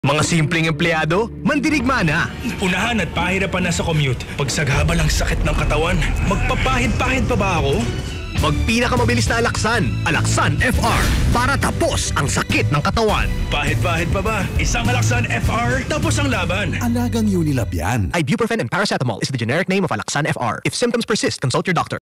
Alaxan FR Radio Commercial 2021-2022 22s (Non-Celebrity Version)
A Non-Celebrity-revised Radio Commercial for Alaxan FR in Filipino. Firstly aired in 2021.

DRPS Two 1210: Alaxan FR Radio Commercial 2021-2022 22s (Non-Celebrity Version)